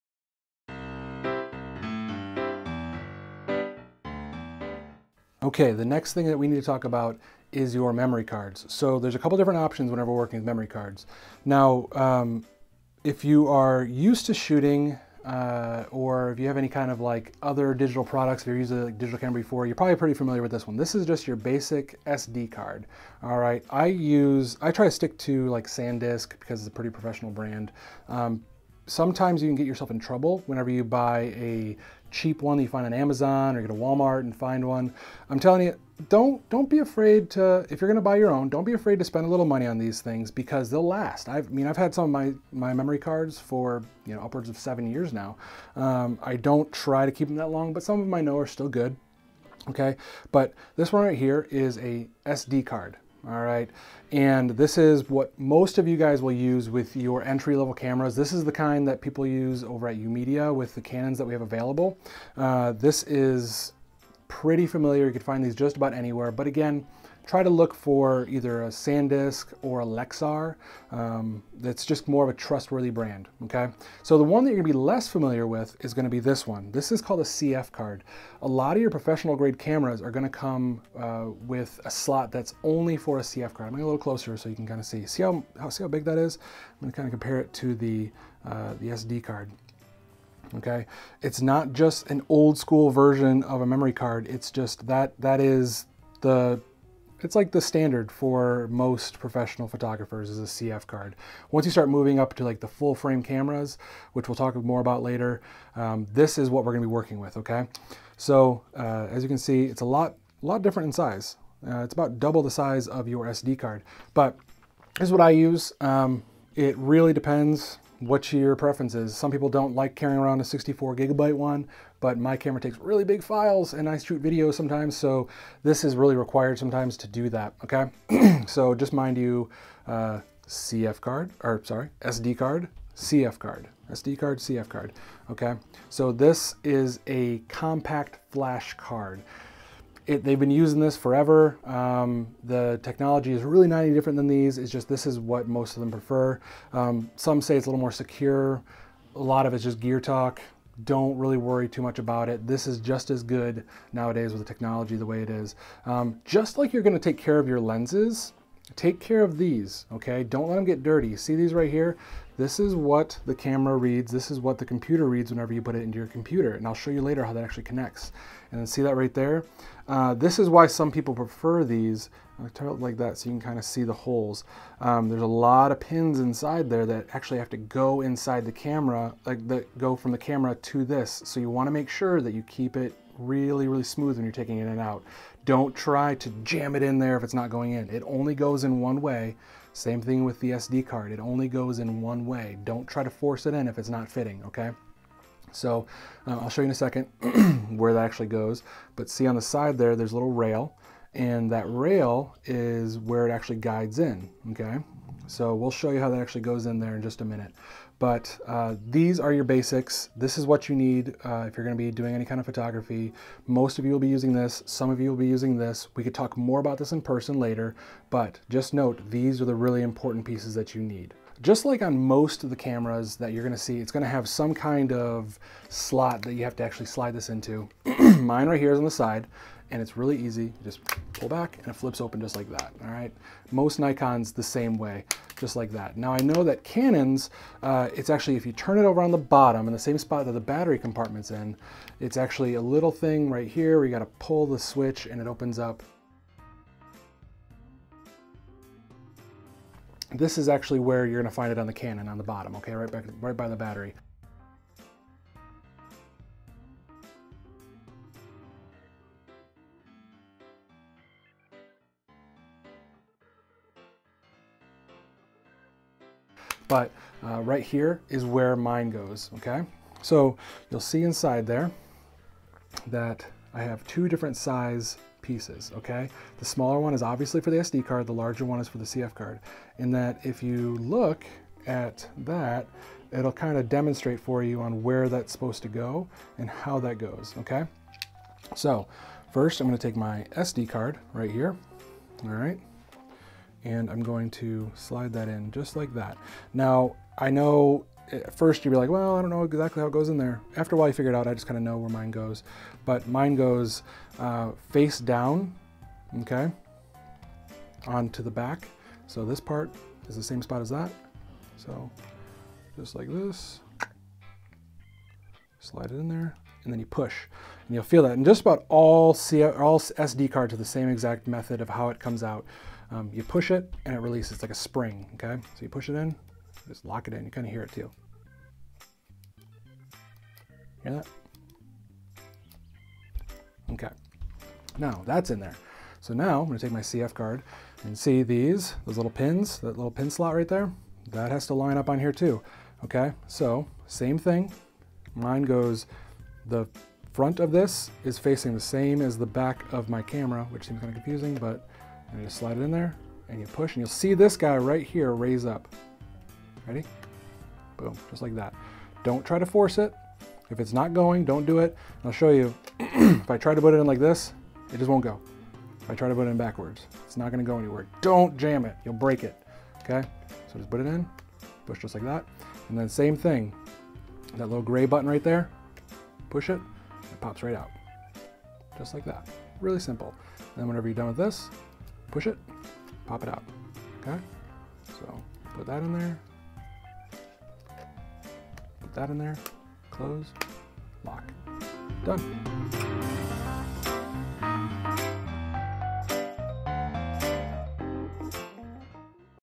LRNG - Intro to Photography - XP 1.5: Memory Cards - SD & CF
Memory cards seem pretty straight forward right? Well slow down. Seriously. Slow down. Jamming that memory card in the camera could do some serious damage to the card as well as your camera.

This video will explain the difference between an SD and a CF card as well as how to handle them.